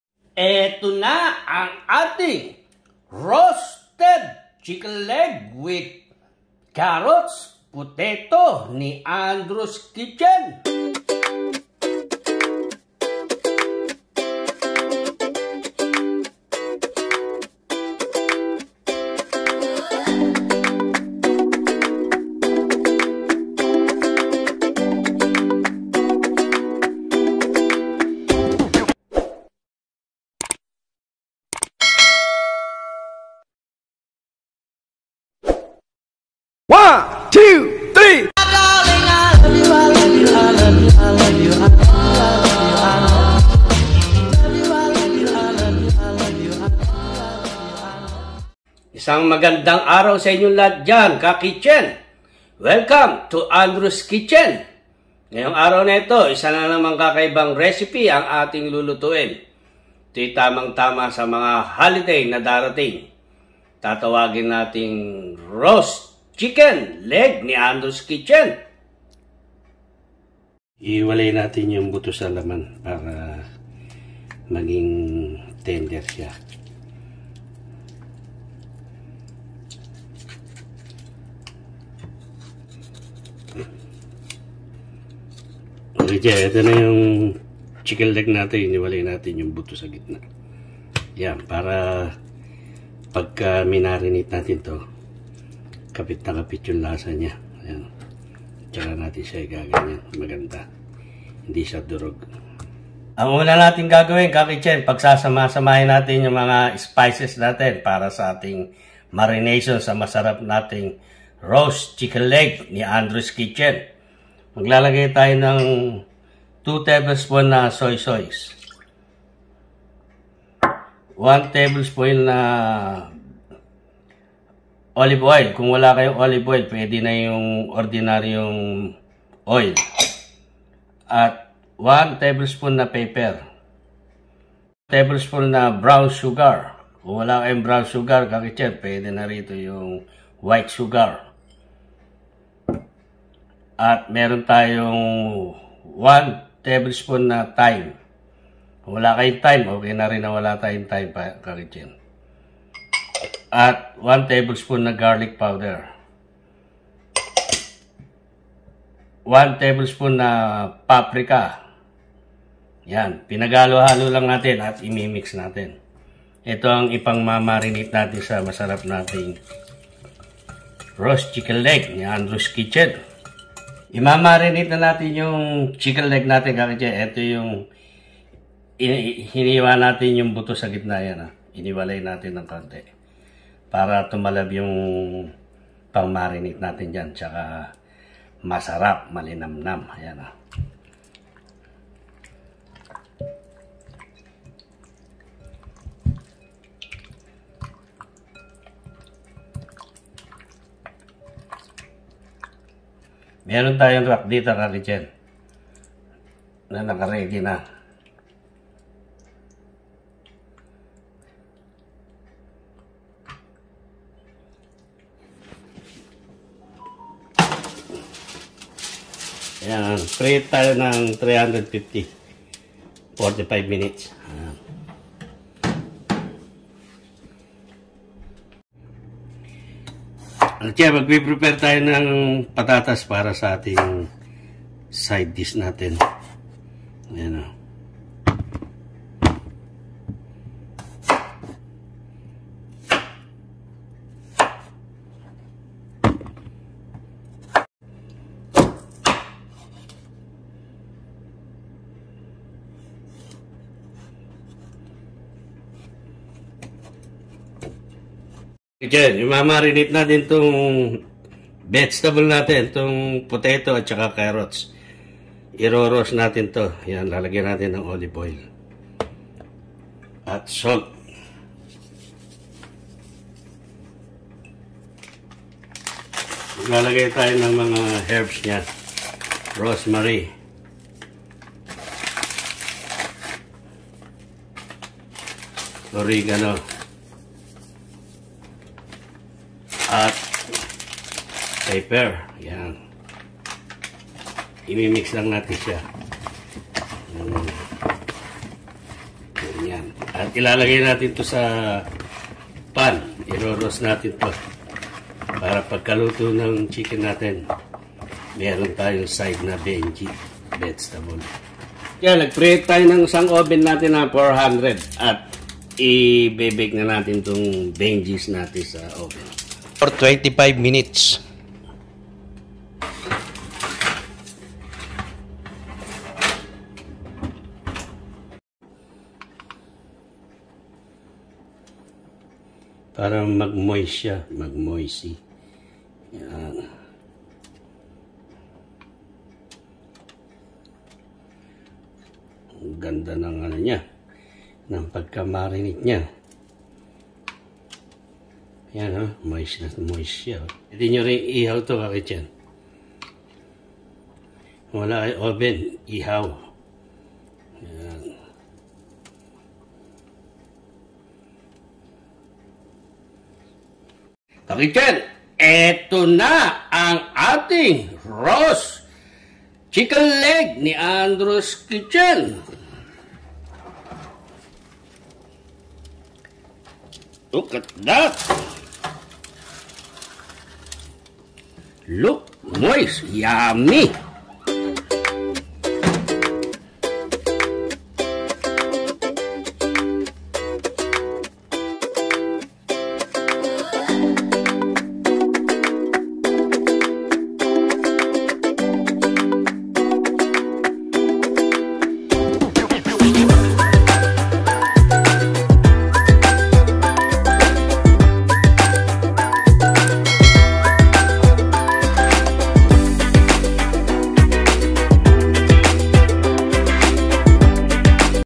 PERFECTLY ROASTED CHICKEN WITH CARROTS AND POTATO!!
PERFECT NA PERFECT TALAGA ITONG RECIPE NA TO FOR NOCHE BUEÑA!! KAYA ANO PA ANG INAANTAY MO?? SUBUKAN MO NA FOR THIS CHRISTMAS!

Chicken legs
2 tbsp of soy sauce
1 tbsp Olive Oil
1 tbsp pepper
1 tbsp brown sugar
1 tbsp thyme
1 tbsp garlic powder
1 tbsp paprika
Potatoes
Carrots
Oregano
Salt
Pepper